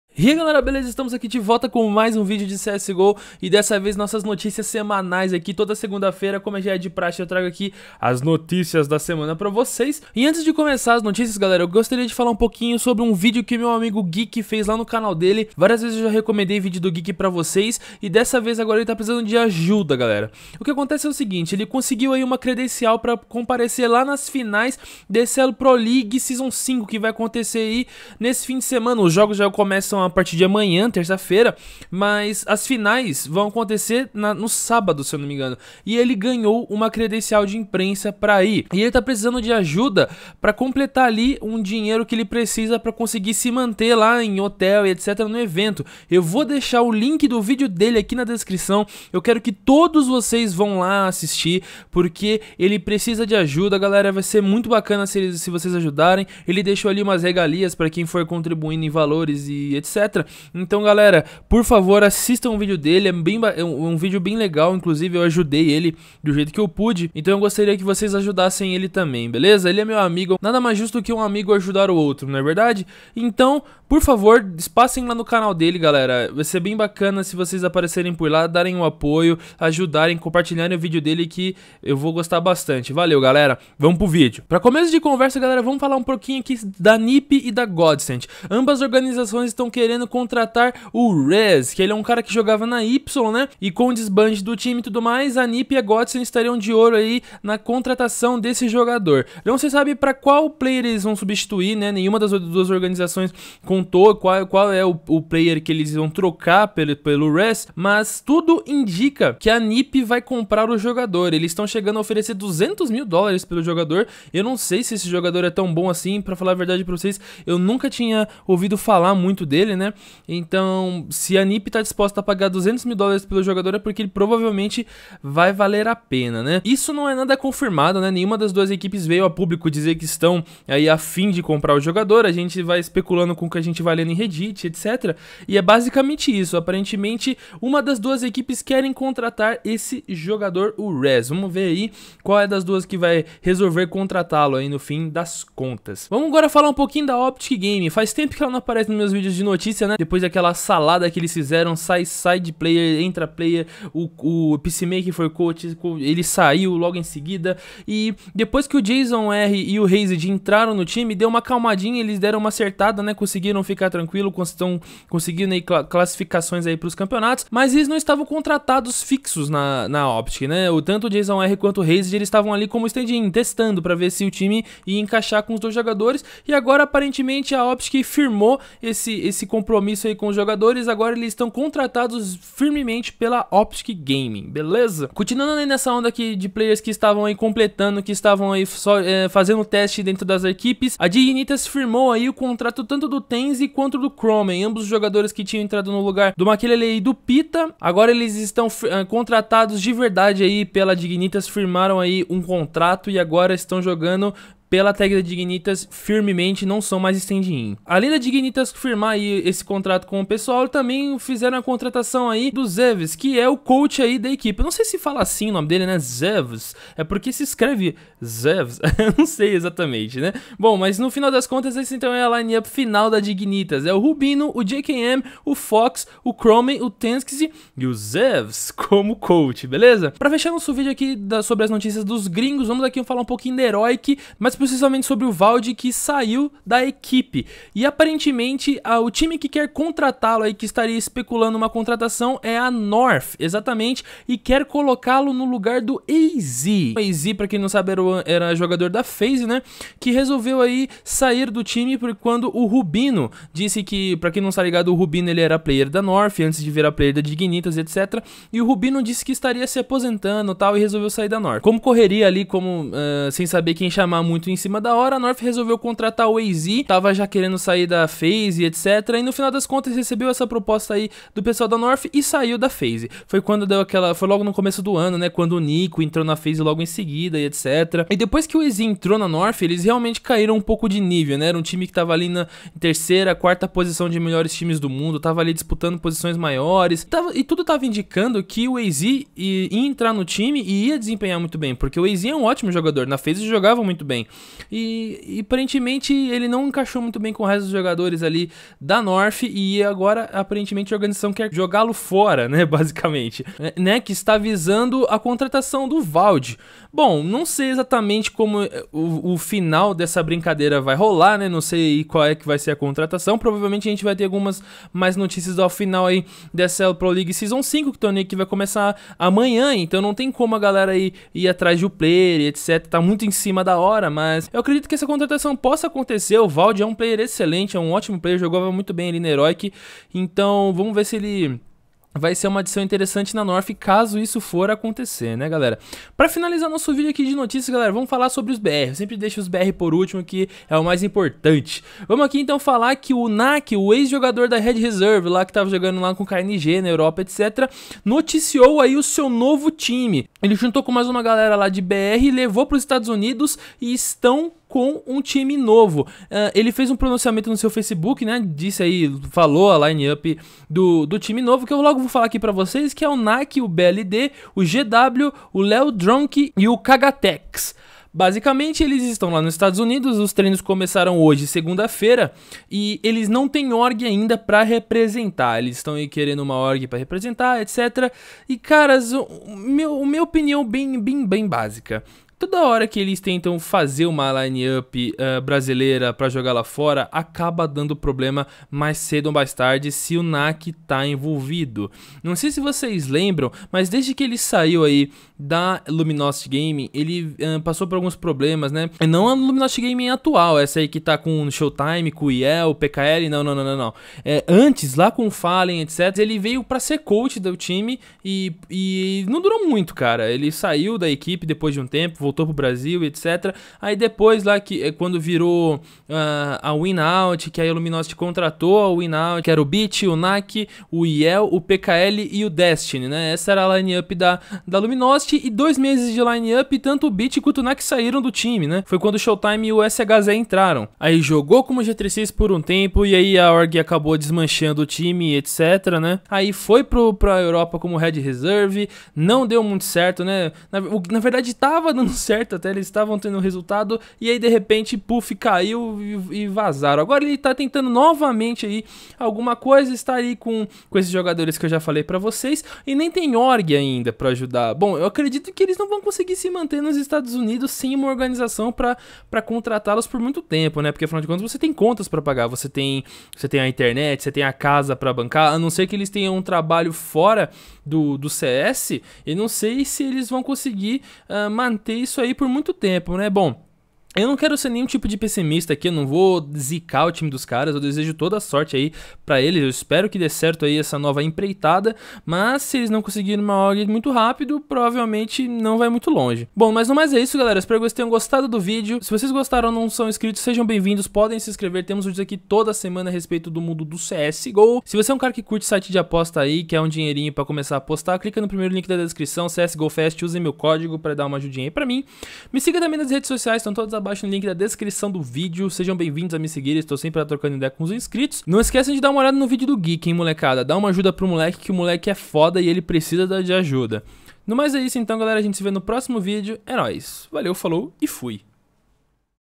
Nak de volta aos EUA com novo time, mudanças na Dignitas e mais! - Notícias CS:GO
---ABRA AQUI---

Dê um Like se você gostou e compartilhe pra ajudar na divulgação.

Vlw por assistir : )